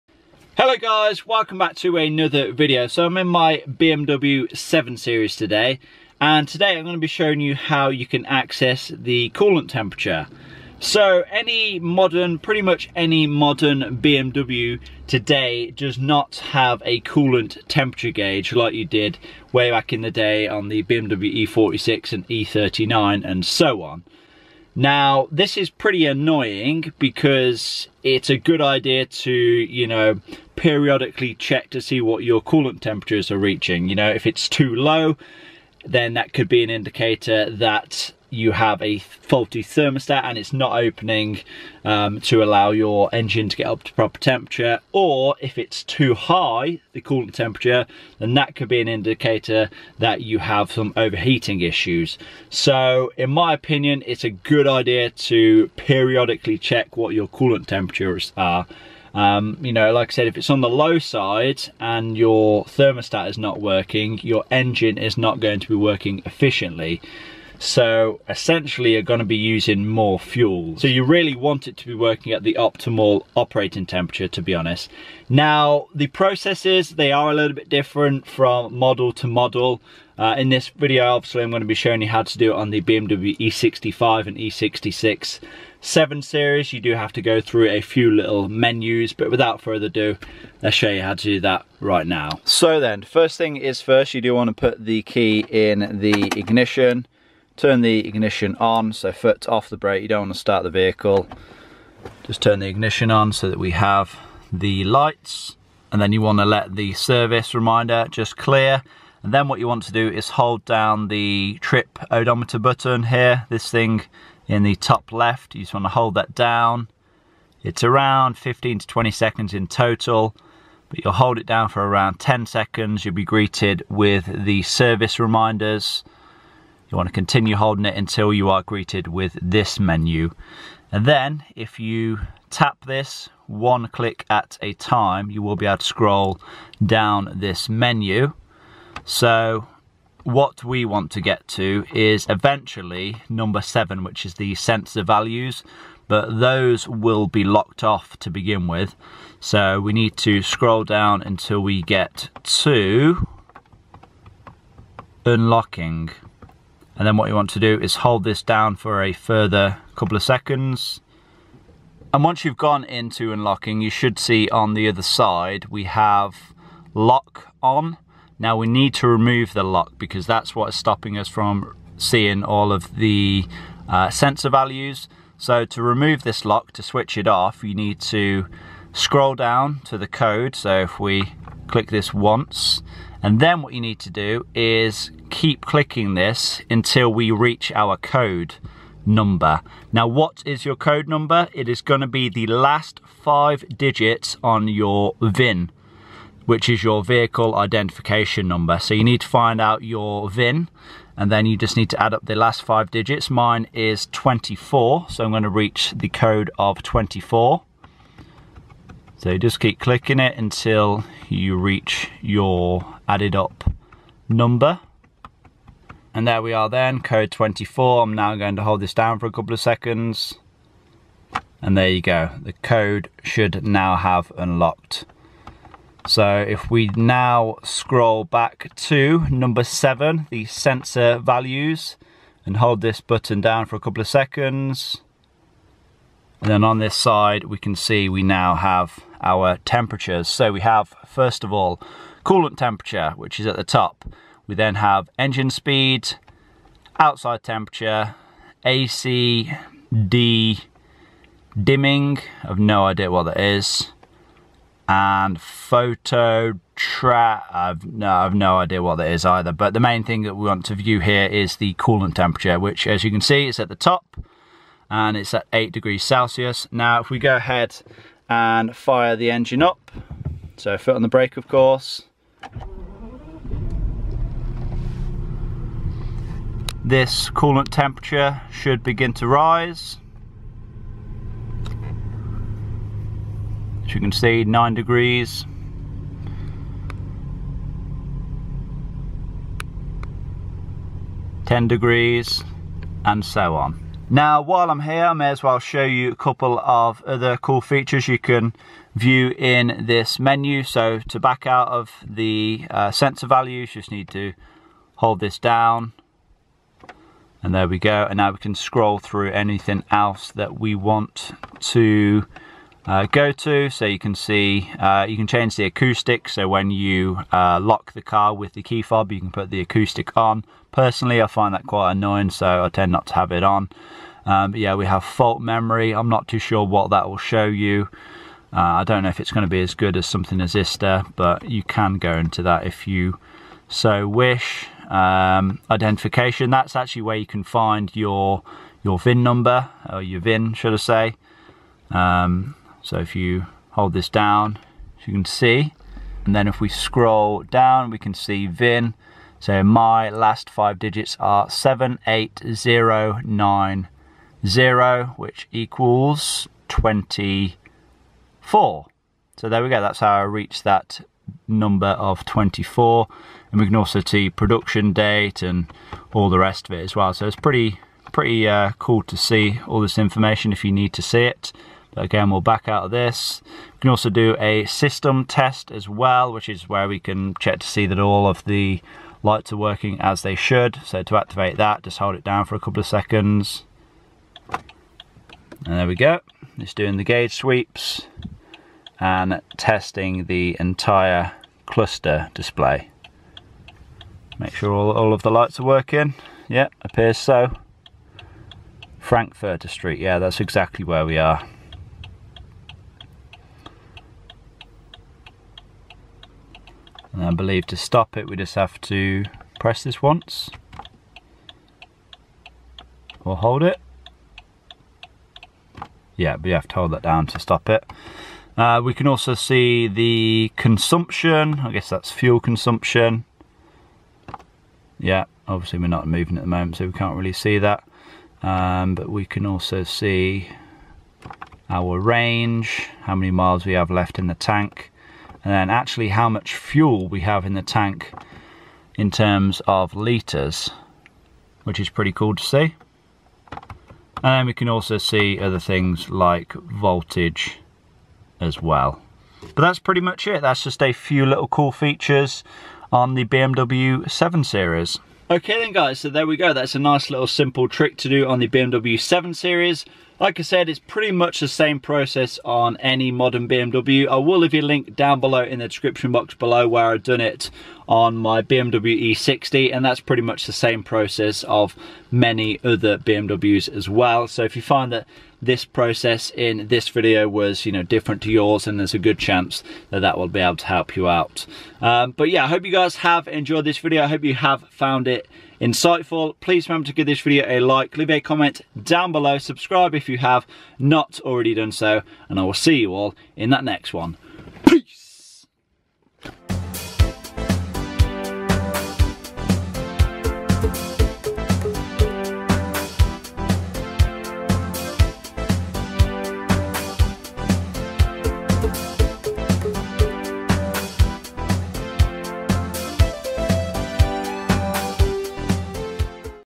BMW 7 Series Secret Menu & Coolant Temperature
How to view BMW 7 series secret menu and coolant temperature. Check this if your car has poor MPG or bad gas mileage.

Small Impact Wrench
Big Impact Wrench
Sealey Impact Socket Set
Torx Socket Set
Female Torx Socket Set
Torque Wrench